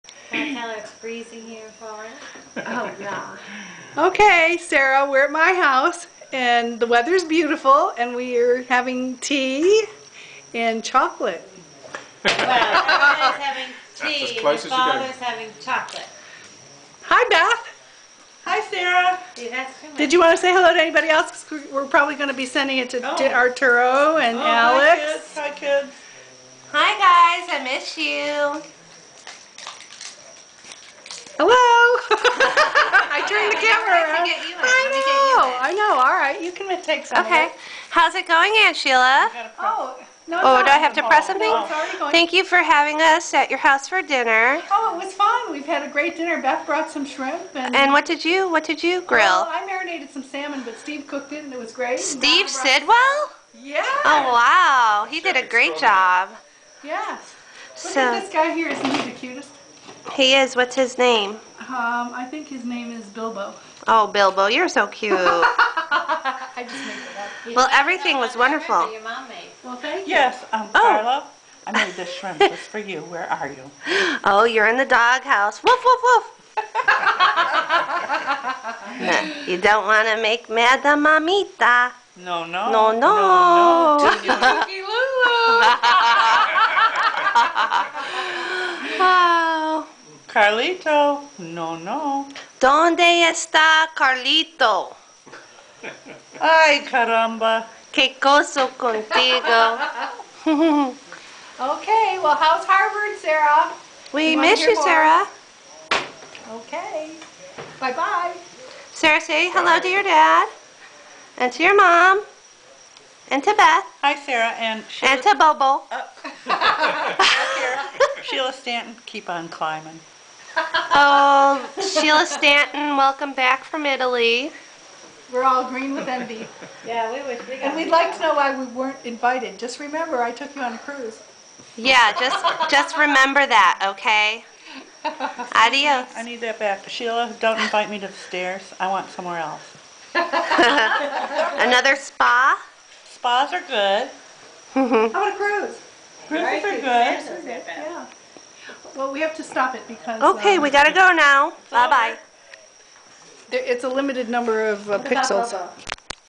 Dinner at Sheila's
Donna & Steve Beth & Cathy & Sheila enjoy salmon and shrimp for dinner.